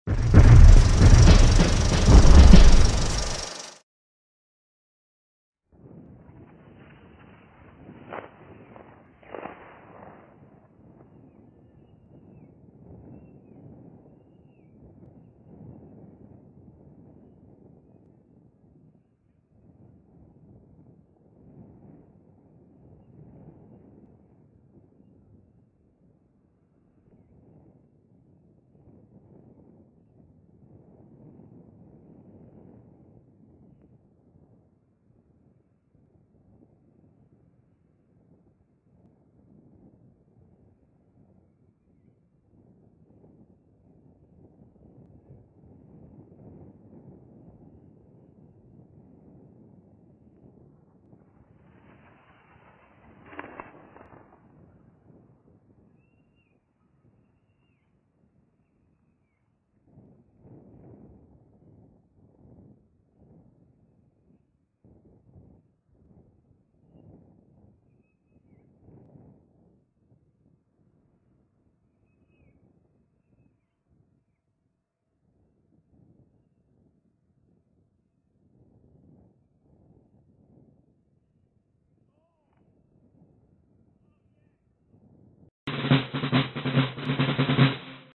US Army and Air Force coordinate to drop 500 LB BOMB on known enemy tactical position
U.S. Army soldiers from the 4th Stryker Brigade Combat Team, 2nd Infantry Division coordinates a U.S. Air Force B-1B Lancer aircraft to drop 500-pound Joint Direct Attack Munition JDAM bombs on a known enemy tactical position.